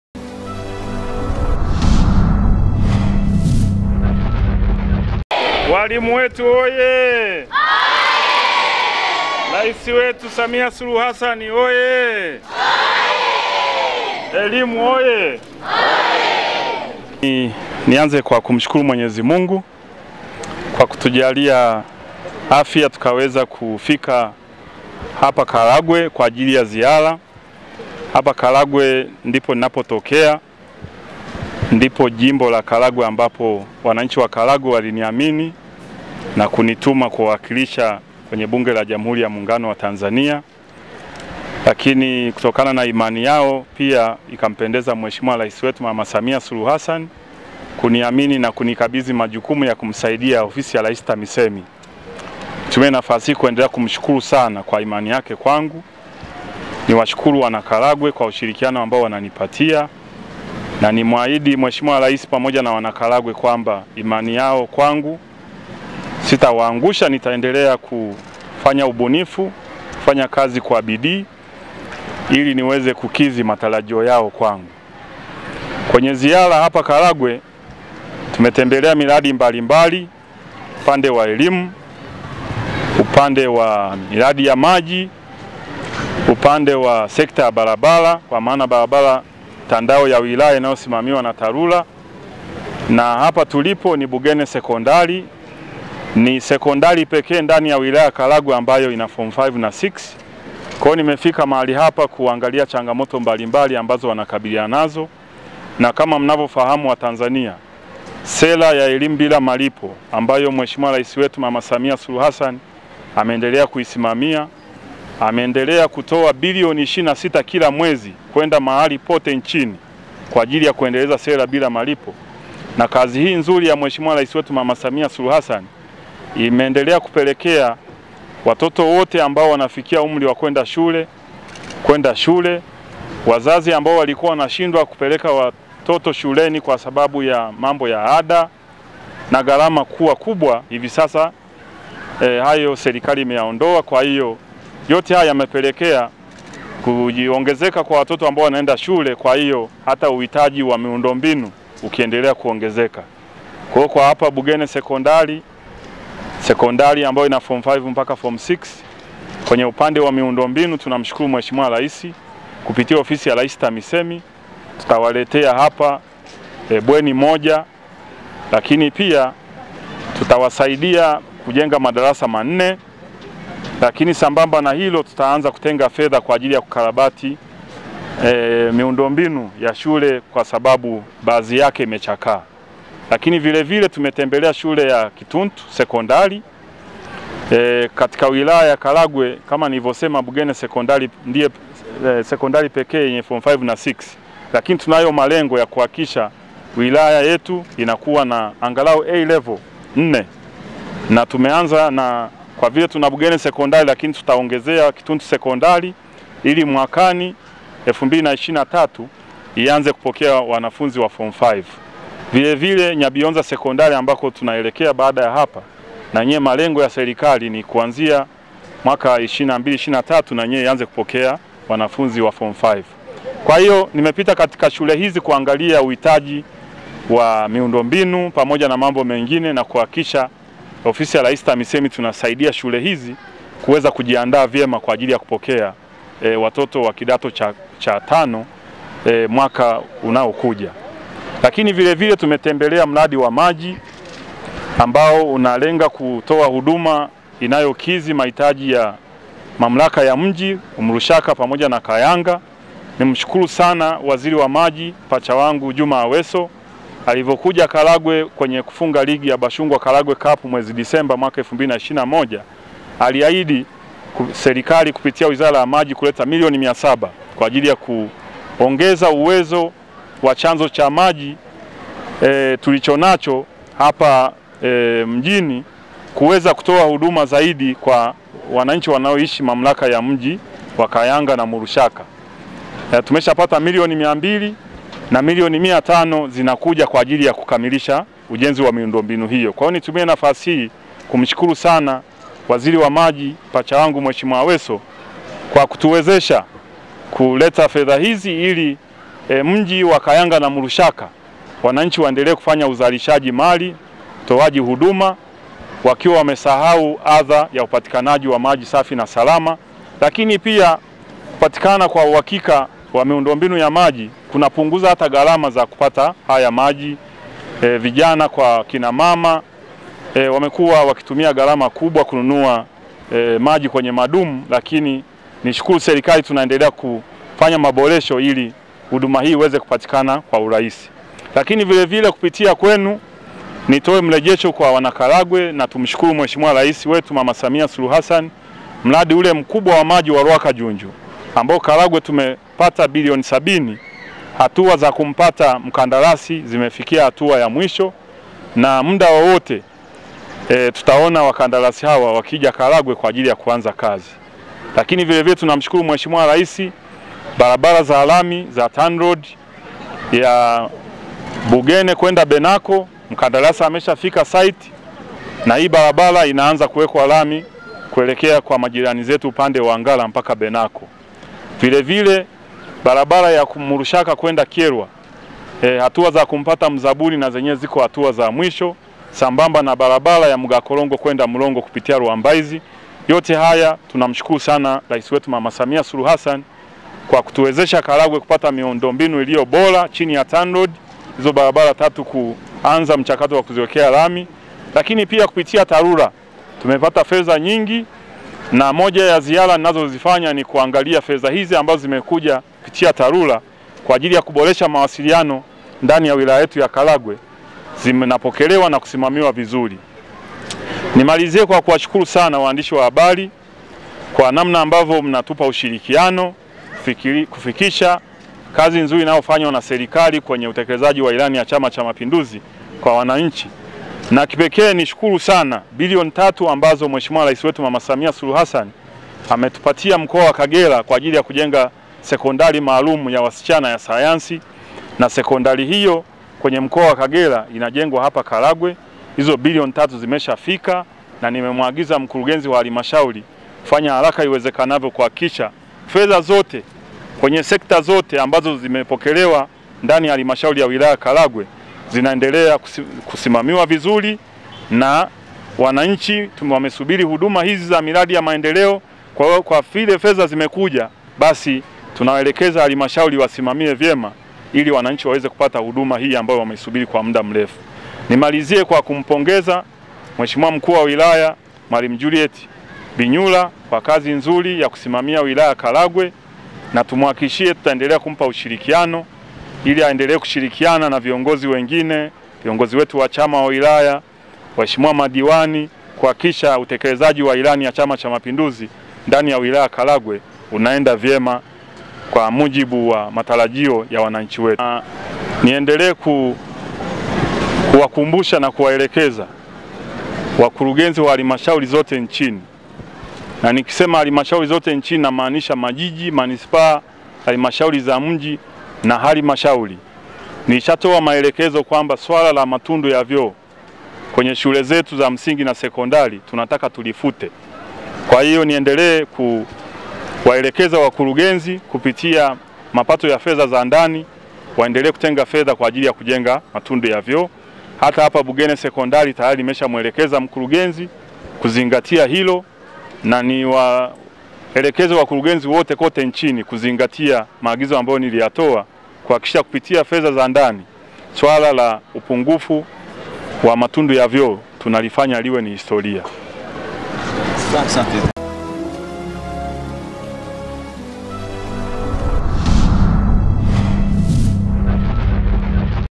BASHUNGWA: BILLION 3 KUJENGA SHULE YA WASICHANA KARAGWE
waziri wa OR-TAMISEMI Innocent Bashungwa na mbunge wa Jimbo la Karagwe amesema kuwa zaidi ya Tsh Bill. 3 zinatarajiwa kujenga shule ya secondari ya wasicha ya mkoa wa kagera itakayo Jengwa katika halmashauri ya Wilaya ya Karagwe.